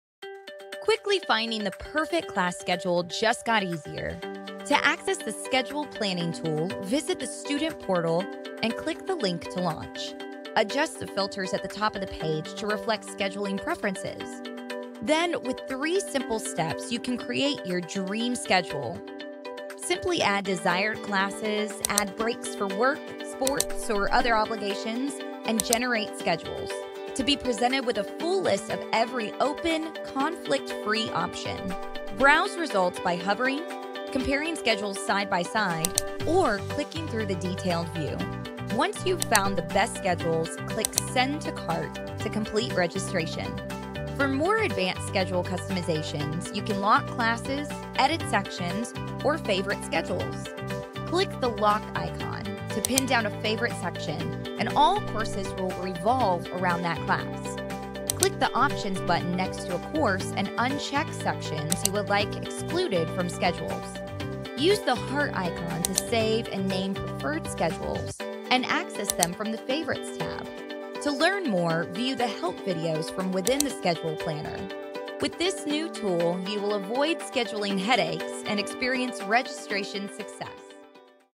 UAB Student Schedule Planner
UAB's Student Schedule Planner will allow you to add courses, generate all possible schedule combinations and select the set of classes that fits you best. Add your preferred schedule to your shopping cart ready for your registration window. Add additional schedule options to your favorites to access as needed during the open registration period.